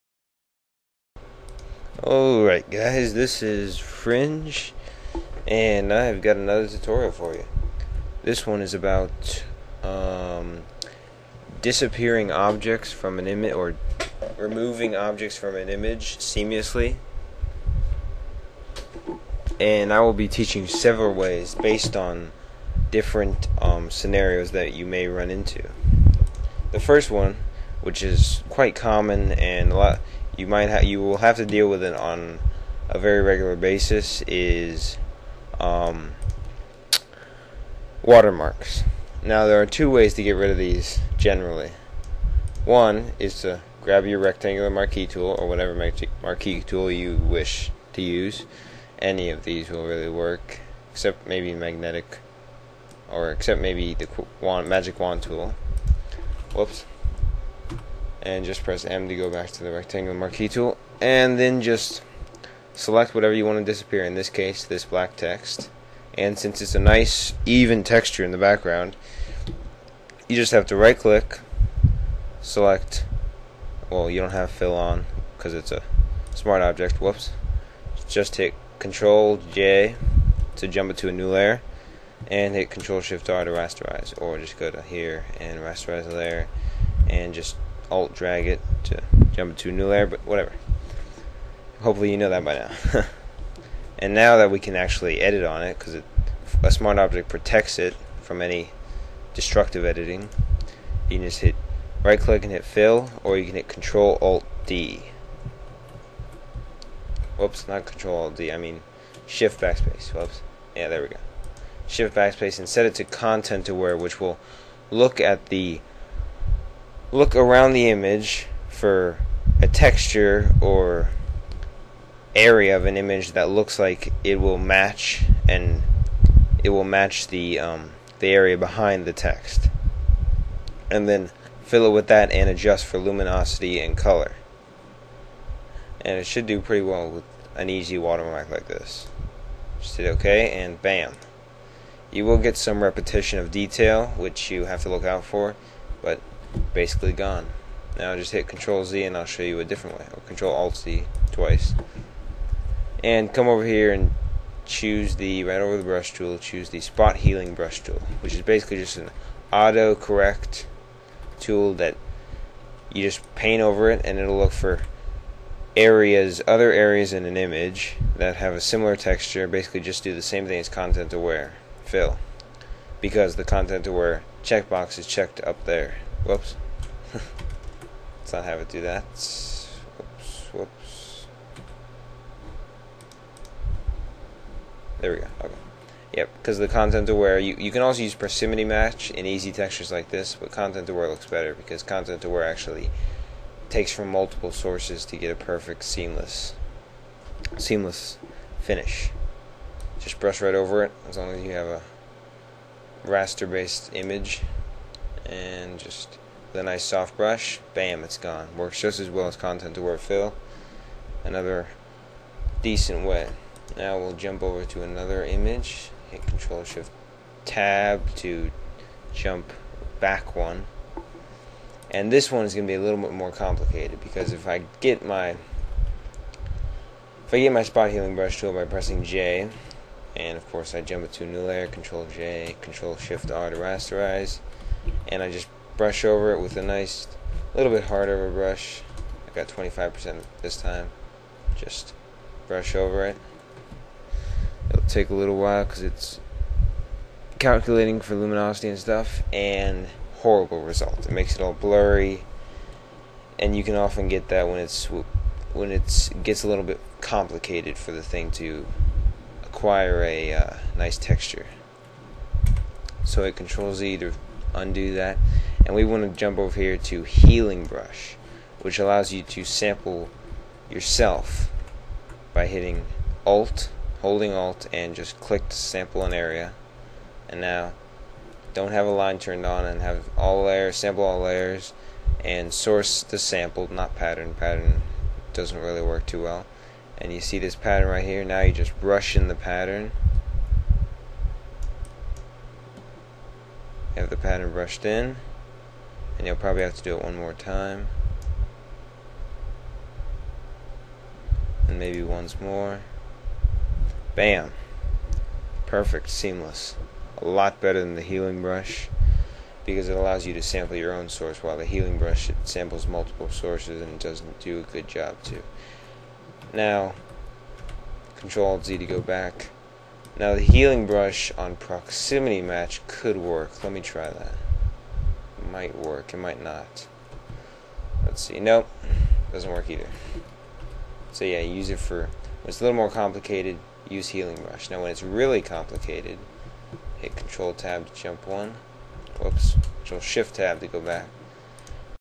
Photoshop Tutorial - Seamless Object Removal Part 1
Seamlessly remove objects using the Healing Brushes, Clone Stamp, and Patch Tool.

Another detailed tutorial for you guys, hope I could help in your future designs. Please leave a comment for the subject of my next tutorial, which I will be doing at least once a week, possibly twice a week.

Program - Adobe Photoshop CS5